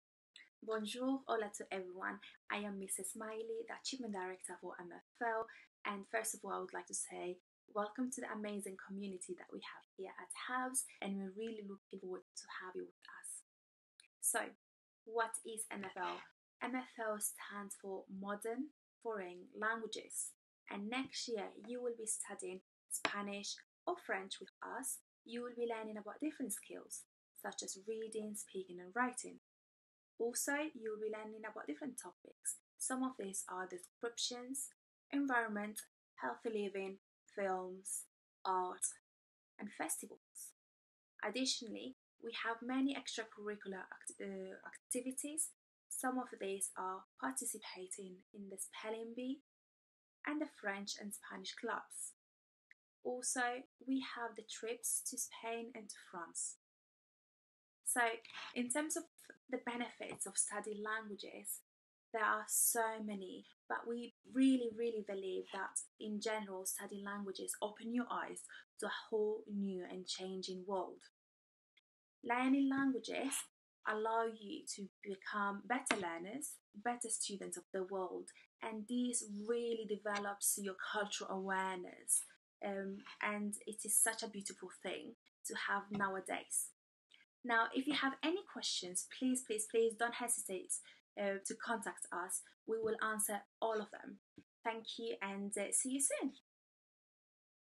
MFL at HABS
Do you love learning new languages? Do you speak multiple languages? Are you excited to learn one if you've never studied this before? Find out more about Modern Foreign Languages here...